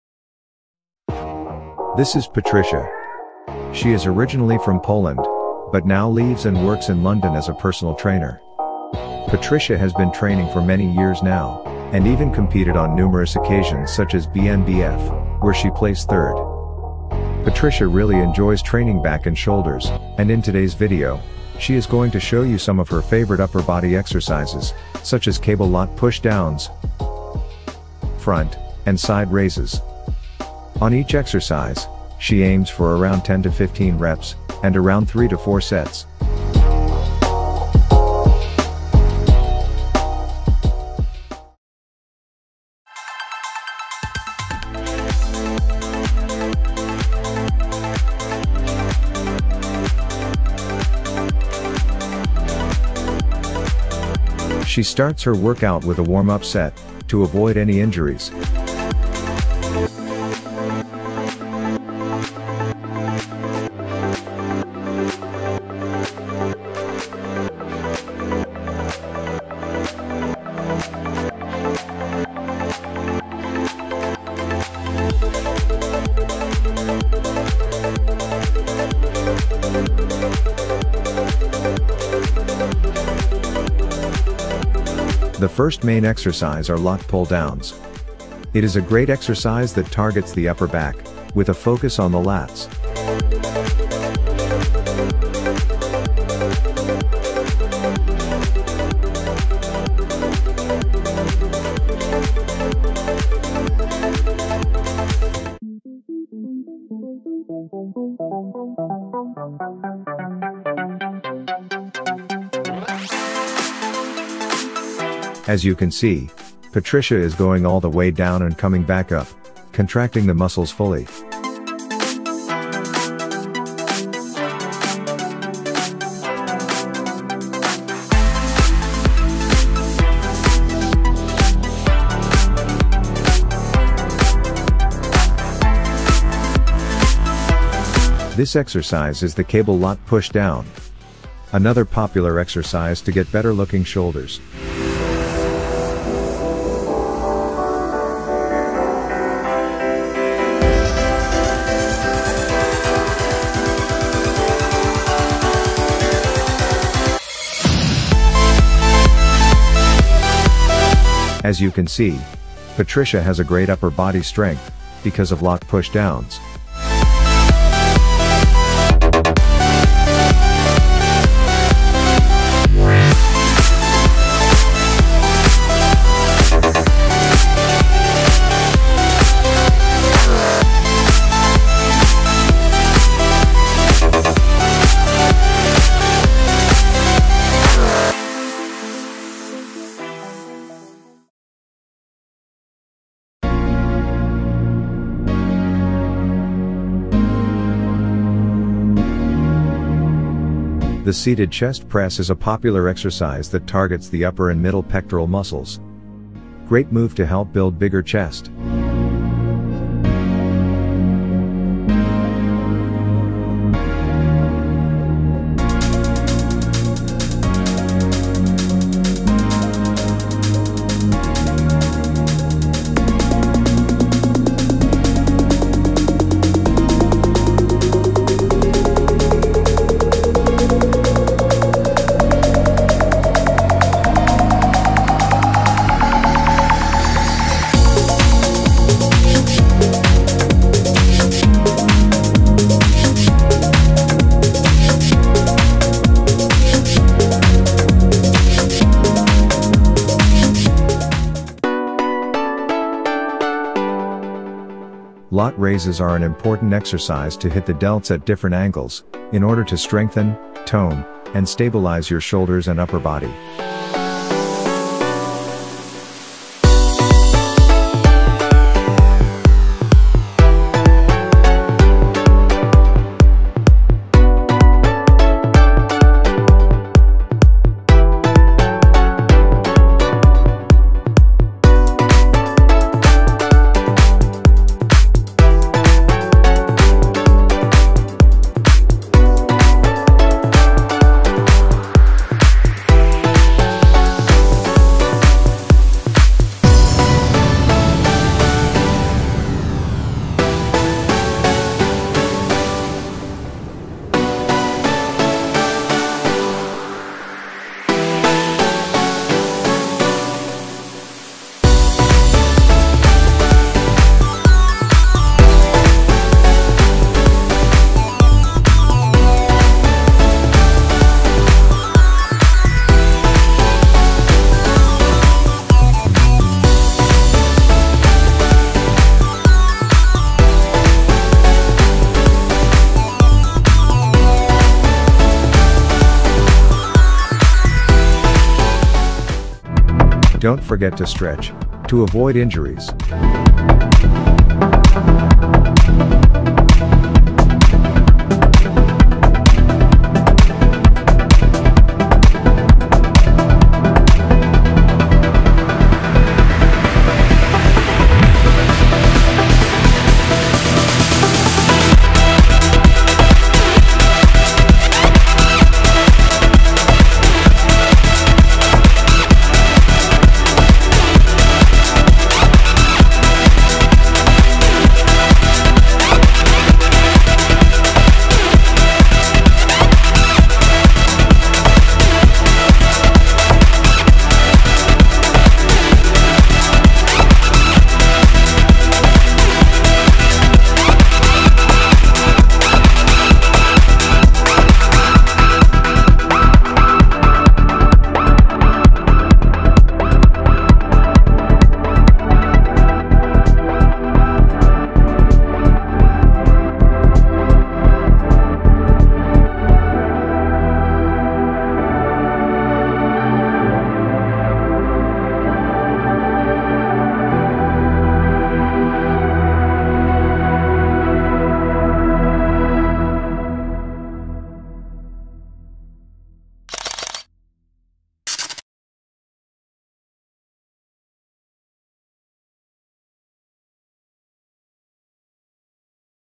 BNBF Competitor Trains Upper Body In a BUSY GYM!
Check out London personal trainer and fitness model Patrycja Pronska
 as she takes you through her upper body workout.